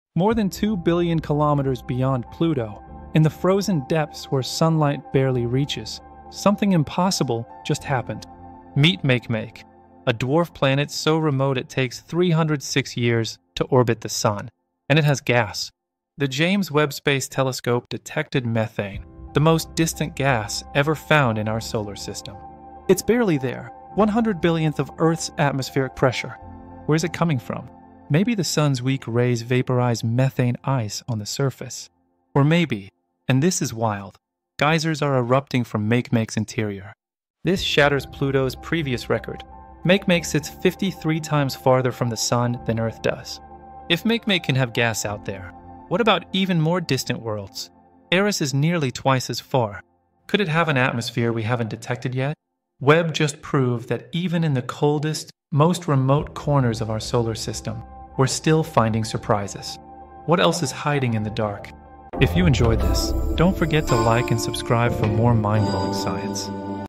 JWST Found Gas Where Nothing Should Exist 🔭
More than 2 billion kilometers beyond Pluto, dwarf planet Makemake just revealed the most distant gas ever detected in our solar system. Using the James Webb Space Telescope, scientists discovered methane gas on a world so remote it takes 306 years to orbit the sun.

☄️ Makemake sits 53x farther from the sun than Earth
🌡️ Surface pressure: 100 billionth of Earth's atmosphere
🔬 First detected by JWST's infrared sensors in 2023
📊 Breaks Pluto's previous record for most distant gas
❓ Source unknown: surface vaporization or underground geysers?

This discovery raises tantalizing questions about even more remote worlds like Eris. What else is hiding in the frozen outer solar system?

SciByte Insights: Complex breakthroughs. Simple explanations. Every day.